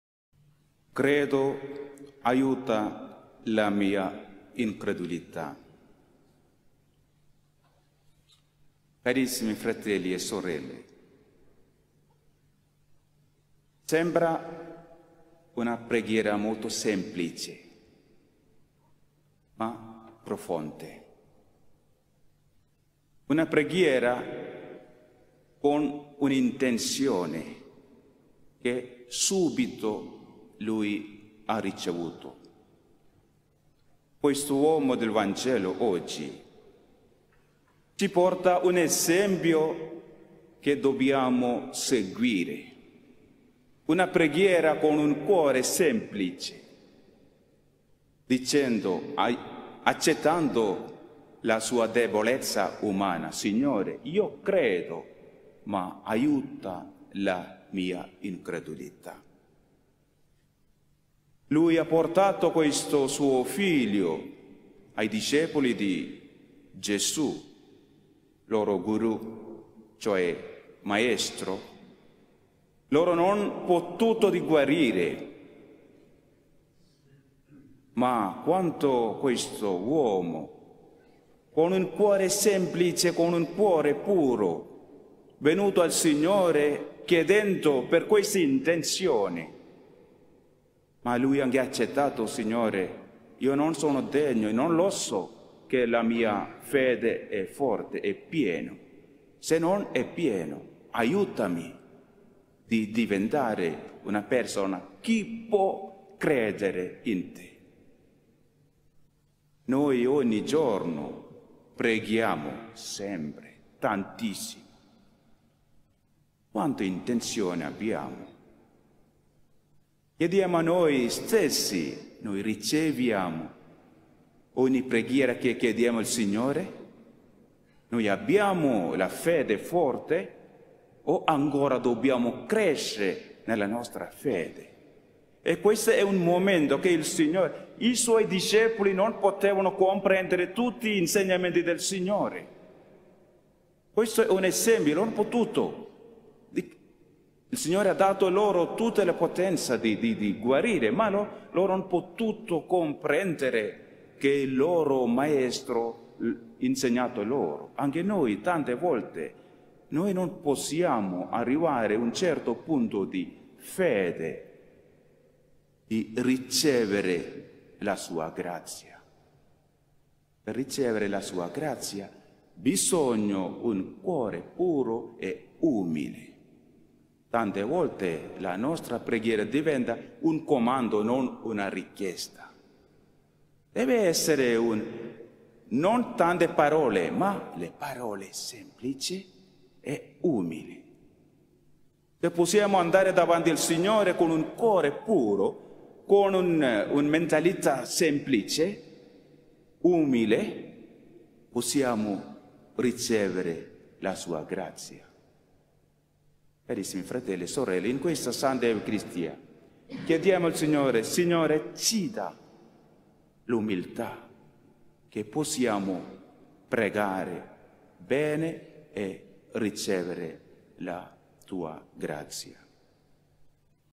20 febbraio 2023 - La preghiera di un cuore umile
L'omelia della s. messa del mattino - Lunedì della VII settimana del Tempo Ordinario.

Immagine di copertina: La preghiera di Francesco fa sgorgare l'acqua dalla roccia, affresco di Giotto e bottega, navata della chiesa superiore della Basilica.